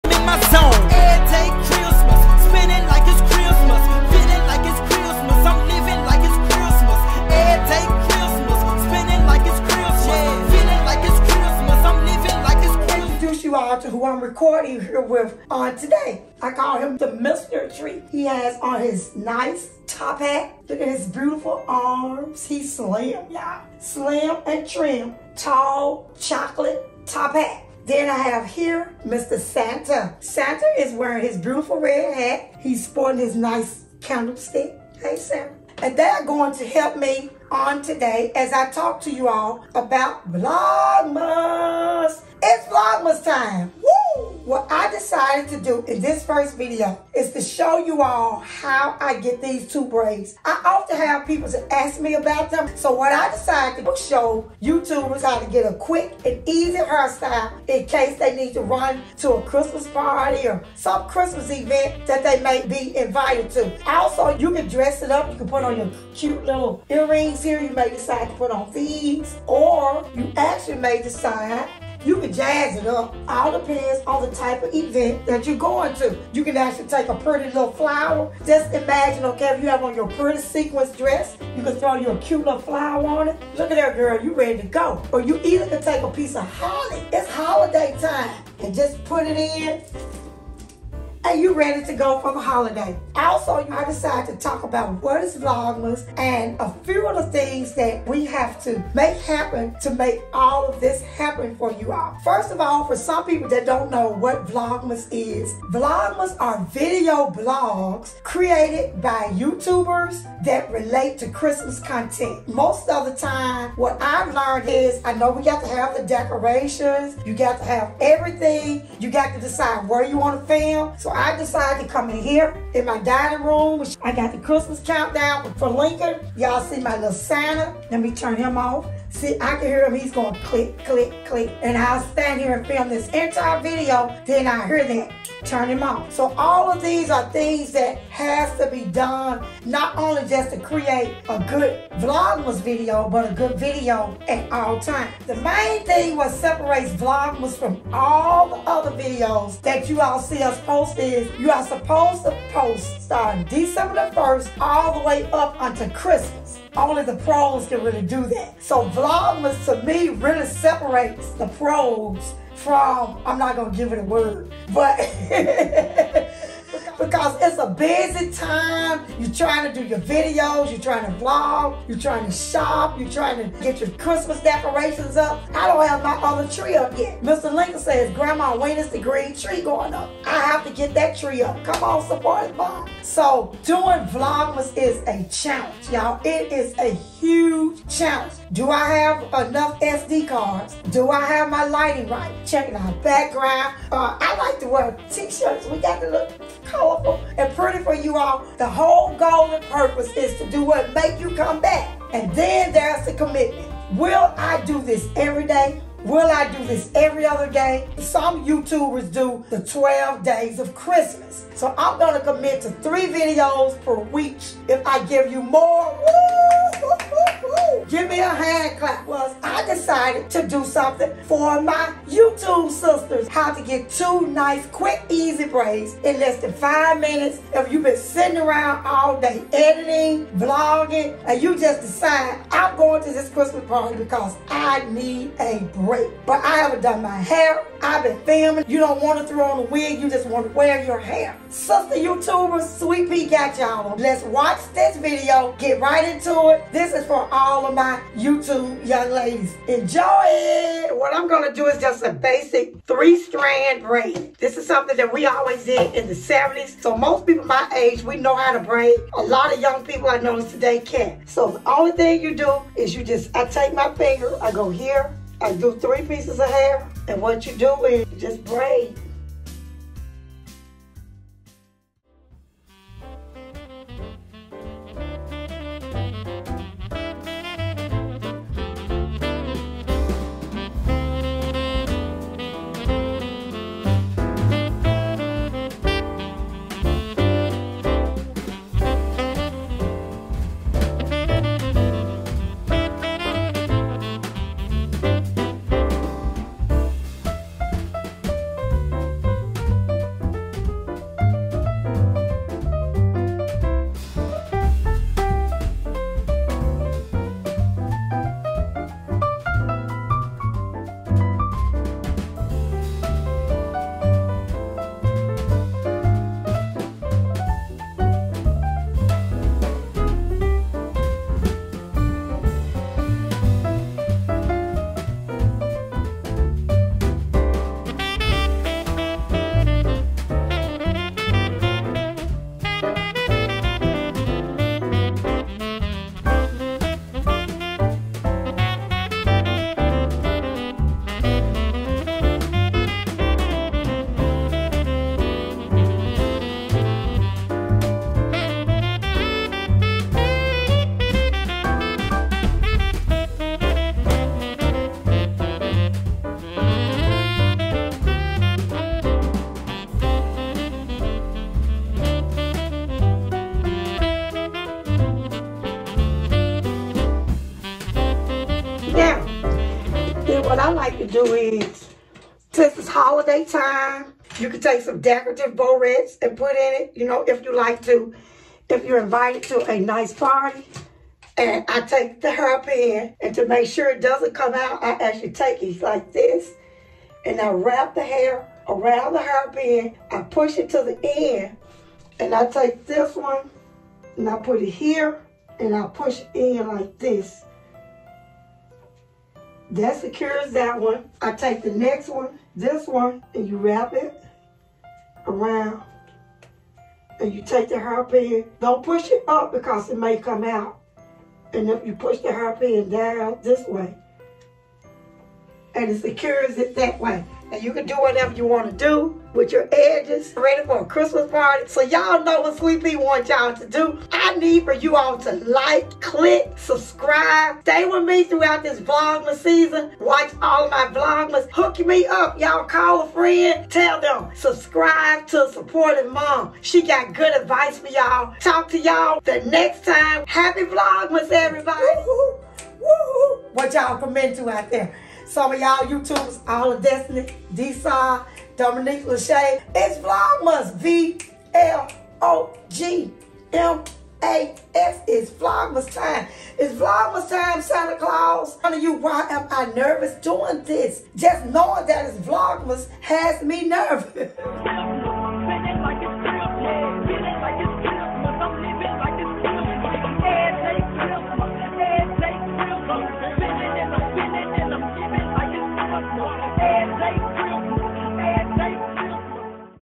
VLOGMAS : Chit Chat | 2 Simple and Easy Braids
In this video I will show you how I braid my natural hair into 2 nice granny braid. Ii is really quick and simple. I use only edge control a brush and a comb for parting. Once the hair is divided into the two sections ; I simply start with 3 sections of hair and add in more hair as I braid it to my scalp and around my face.